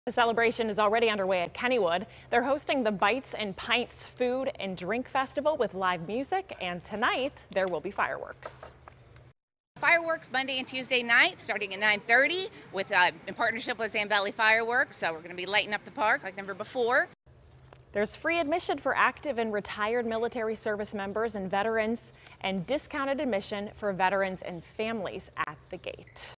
Kennywood hosting 'Celebrate America' events for 4th of July
Fireworks will light up the park on Tuesday night at 9:30 p.m.